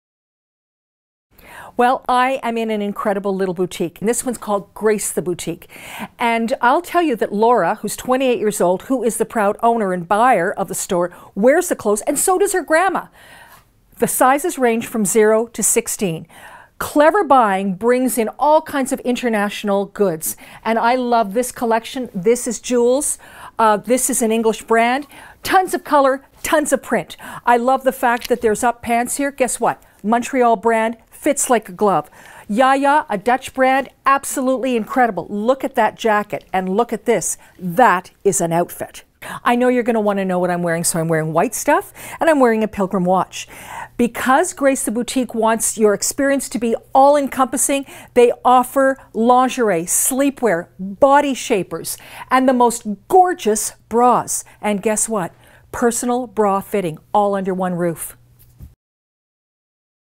Lynn Spence Shops Stratford: Grace the Boutique
Check out Lynn's fourth stop on her Stratford shopping adventure at Grace the Boutique!